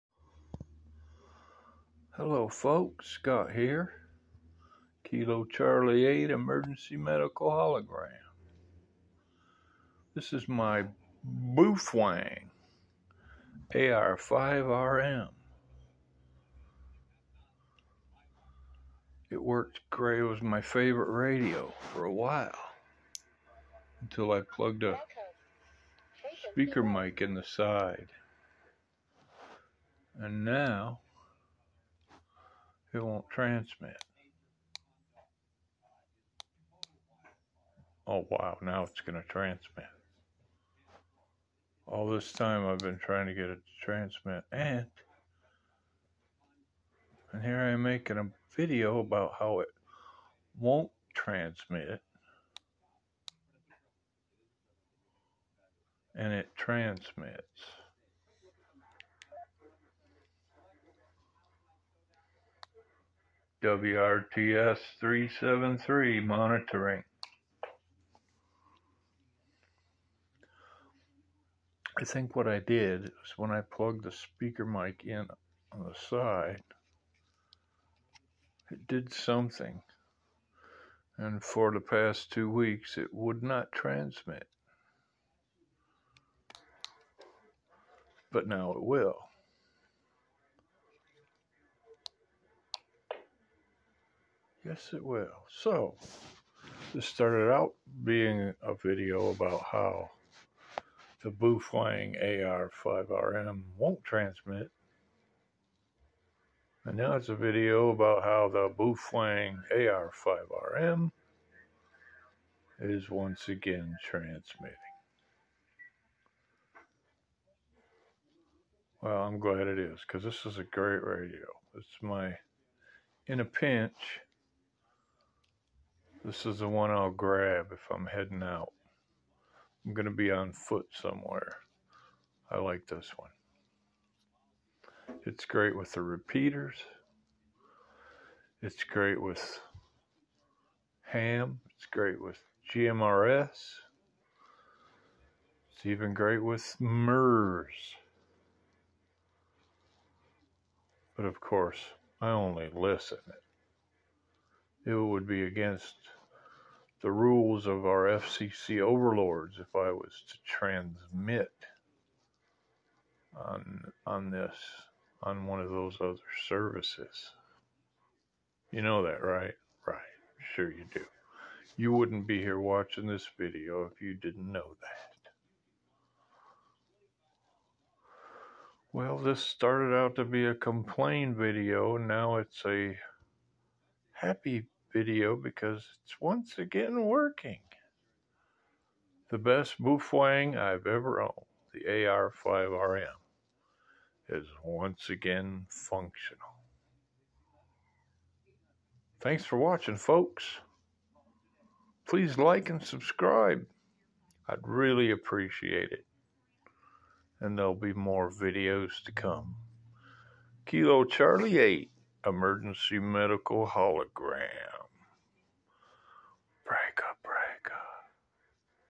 The Baofeng AR-5RM That Wouldn't Transmit Until I Made This Video
After plugging in a Baofeng speaker mic for use as a mobile radio in the car, it wouldn't transmit with the mic, and after removing the mic, it wouldn't transmit at all... Until I made this video!